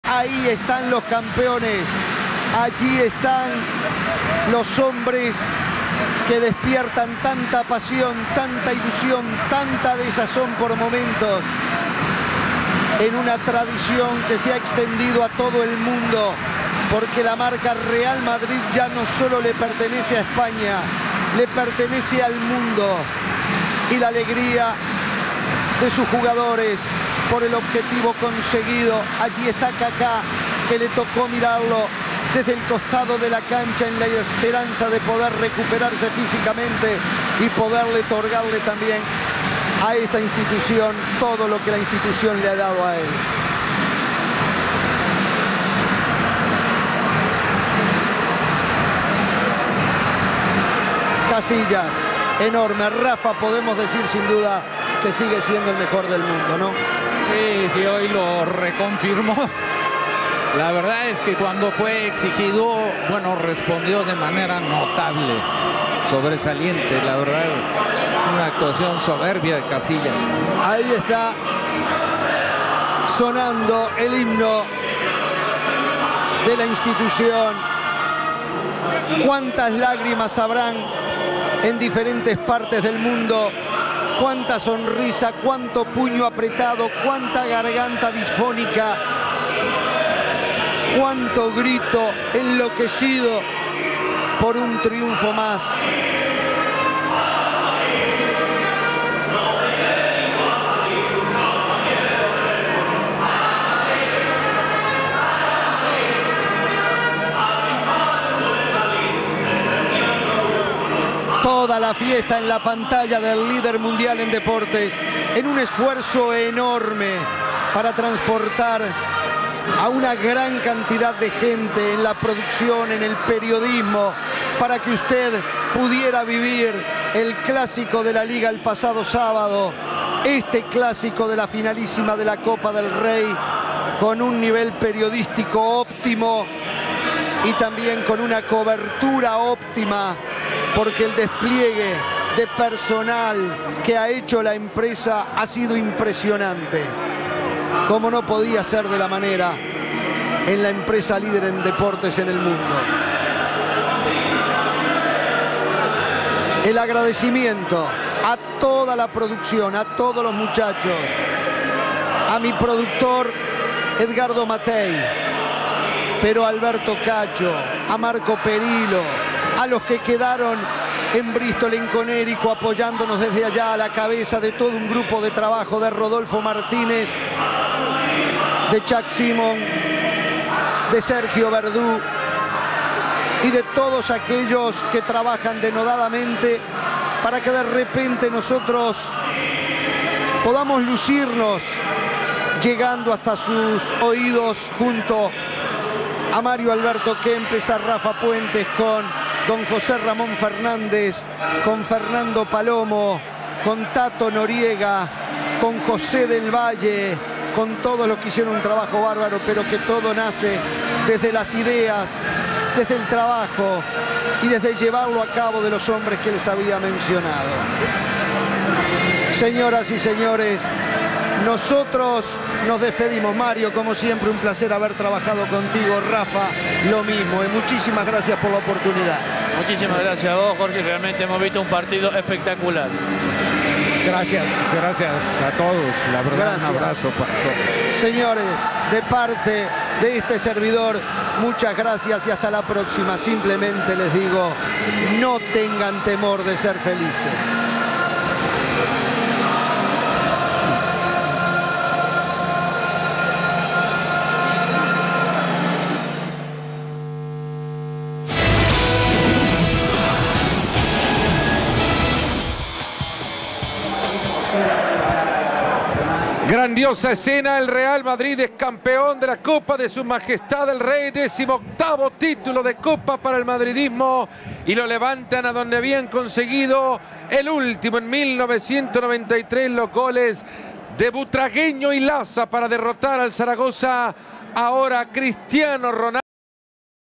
Celebración Real Madrid (Campeones de la Copa del Rey 2010/2011) 2/2
El Real Madrid se proclamó campeón de la Copa del Rey 18 años después tras imponerse por 0-1 al Barcelona en la final de Mestalla. Decidió Cristiano Ronaldo con un tanto en los 12 minutos de la primera parte de la prórroga. Mourinho logra así su primer título como entrenador merengue.